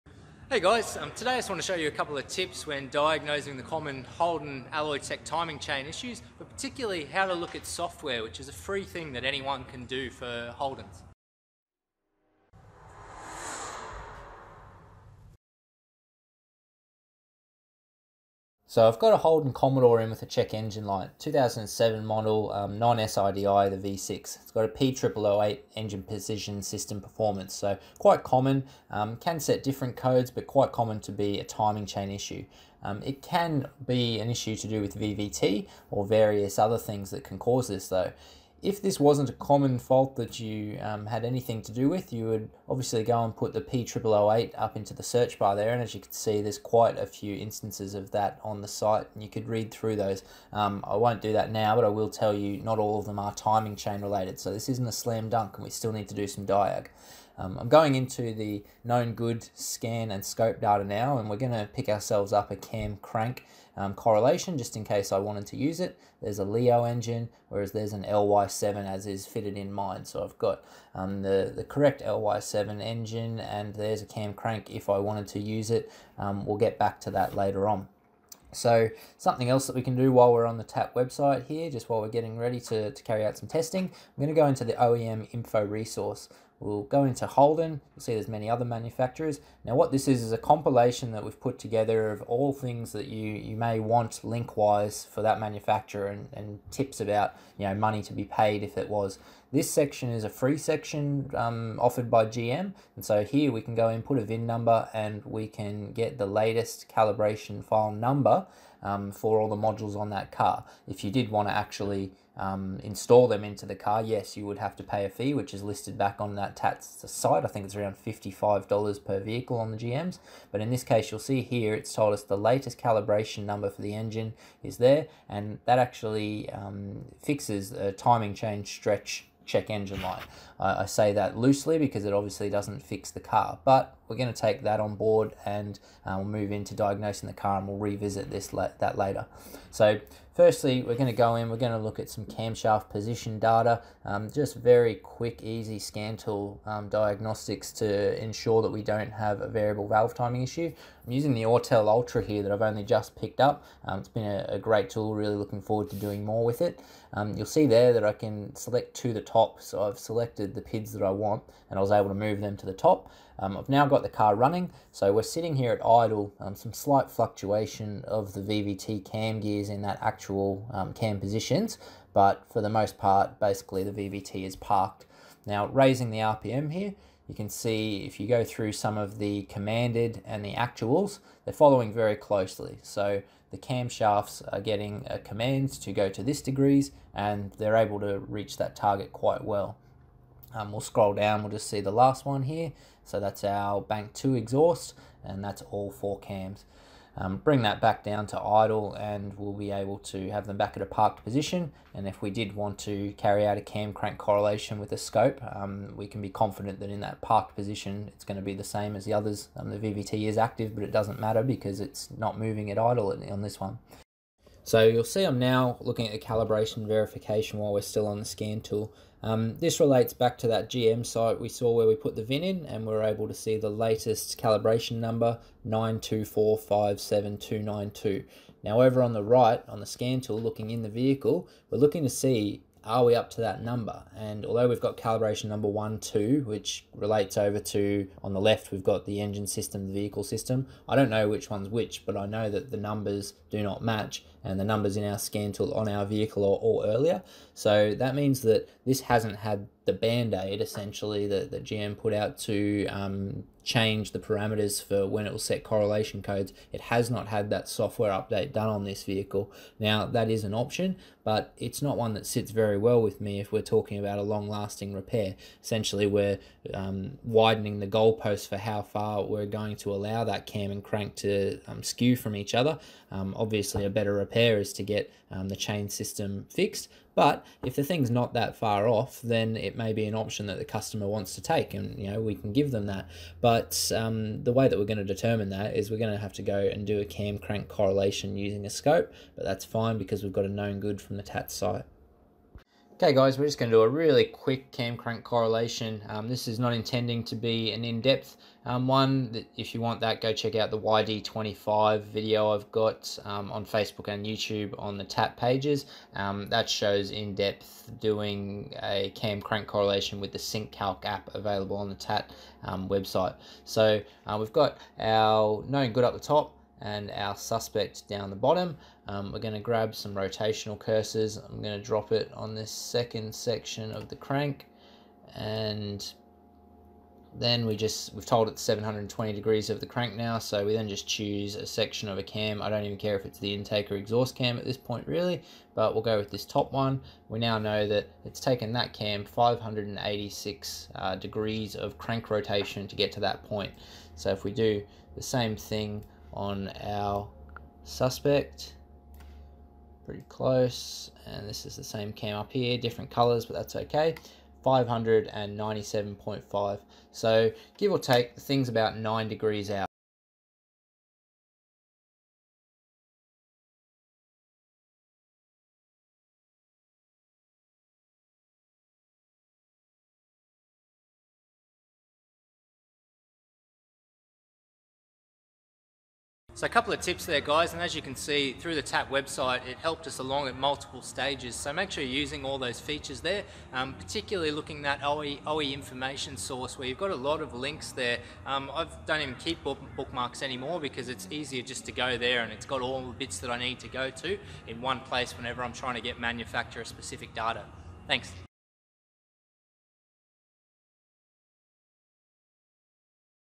VE Commodore P0008 testing
Using all the tools available to you on the TaT website will speed up testing for this common fault.

In this video we focus on utilising the GM Calibration check tool, linked through the TaT OEM Info Resource.